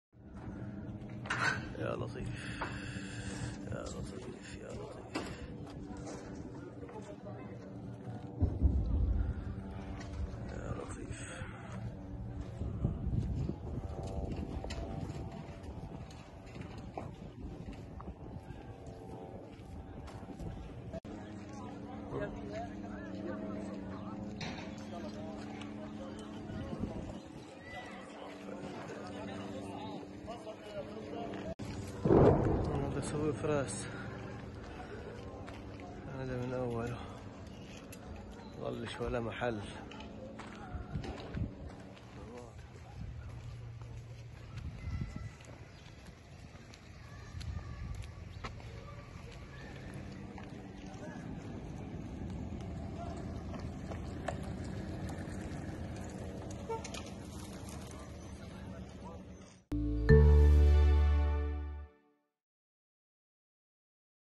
فلسطينيون يسيرون في الشوارع المدمرة بالقرب من سوق فراس في مدينة غزة
فلسطينيون يسيرون في الشوارع المدمرة بالقرب من سوق_فراس في مدينة